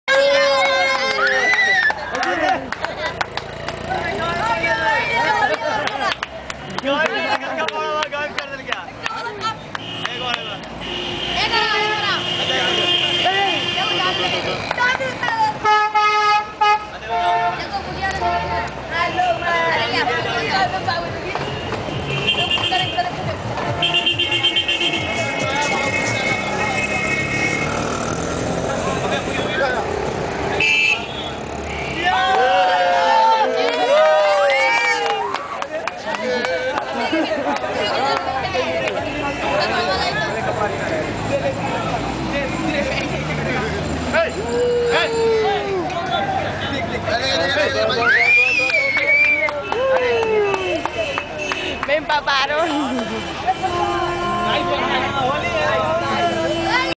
Magic trick in Calcuta -India-
Truquito de magia. Desaparicion de pañuelito en mercado callejero de Calcuta (India)